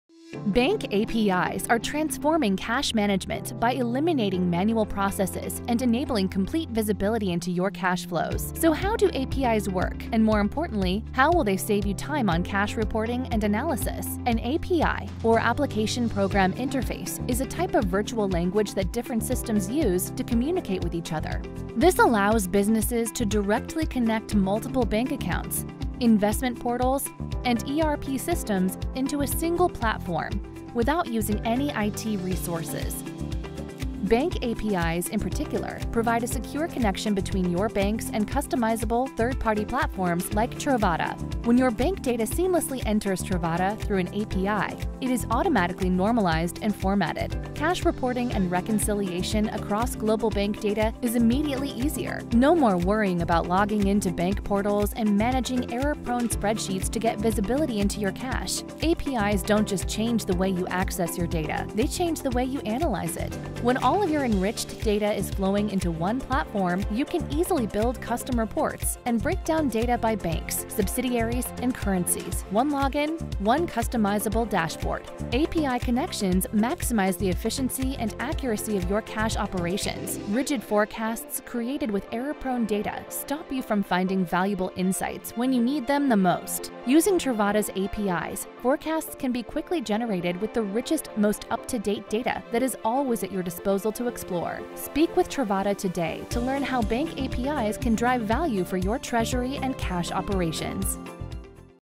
How are Bank APIs Transforming Cash Management Operations?
What is a Bank API? A bank API is a critical element of openbanking which allows third-party companies to build new services for banking customers. These services include data visualizations, flexible methods of categorizing transactions for the unique scenarios of businesses, moving assets between accounts (or different banks or even different currencies), and so on. Banking APIs are at the heart of contemporary and future growth in fintech.

The development of an API allows clients of financial institutions to get enhanced services from specialized software companies.